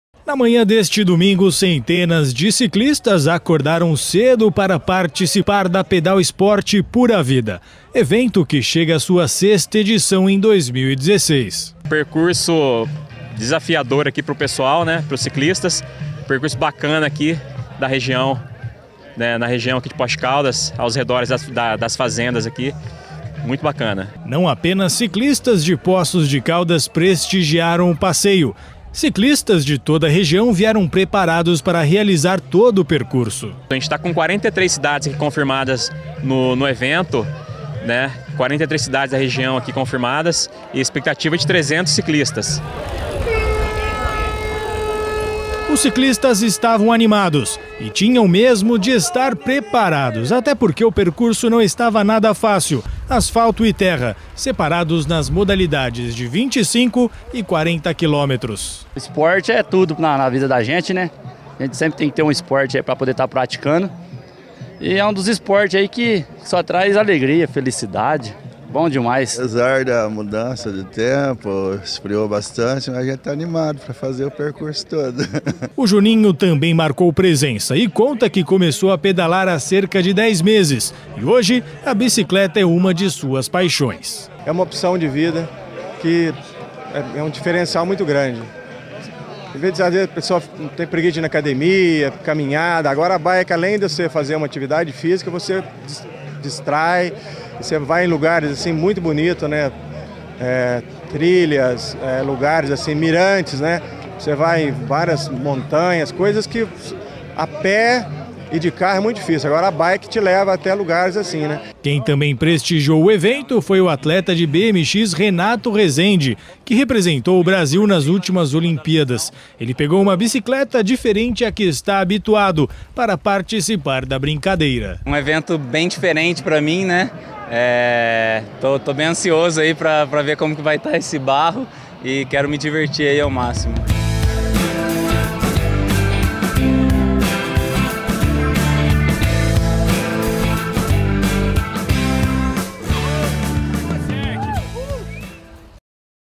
PEDAL SPORT PURA VIDA
Mais de 300 ciclistas participaram do 6º Pedal Sport Pura Vida na cidade.